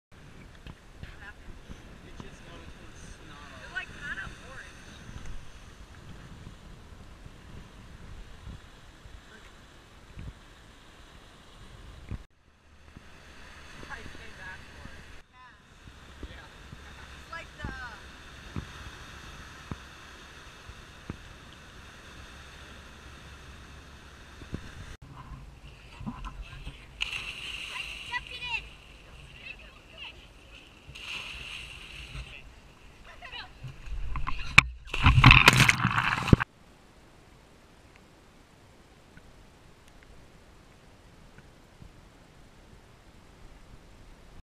Maine Haddock Fishing and Echo Lake
Fishing action along the Maine shoreline along with glimpses of Maine lake life at Echo Lake